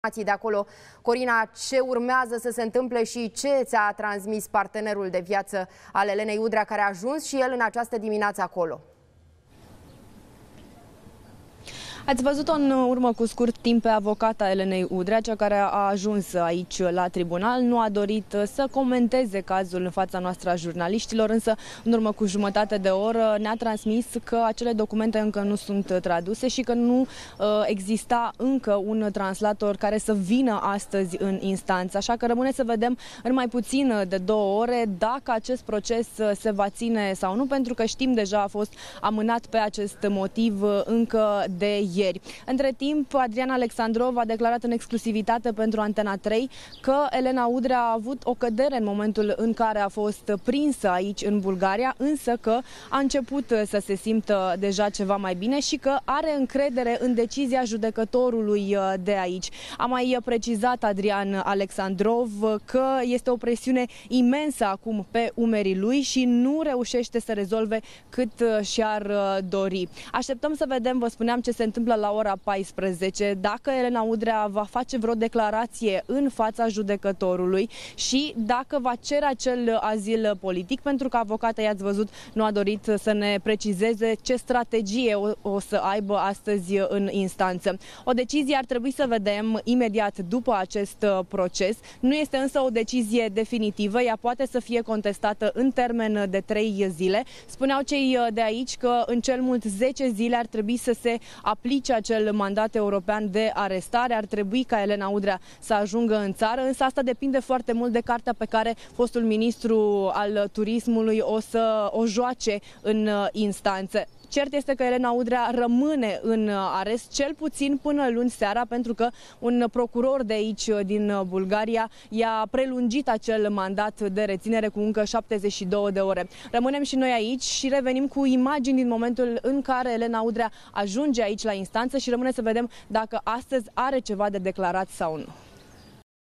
Elena Udrea, cădere nervoasă după ce a fost prinsă în Bulgaria
Până la această oră nu se știe dacă documentele așteptate de magistrații bulgari în cazul Elenei Udrea sunt traduse, dacă aceasta va avea translator astăzi, iar tensiunea resimțită de fostul ministru este uriașă. În mai puțin de două ore, Elena Udrea ar trebui să fie din nou prezentată instanței din Bulgaria, după ce ieri nu a putut fi audiată din lipsa unui traducător.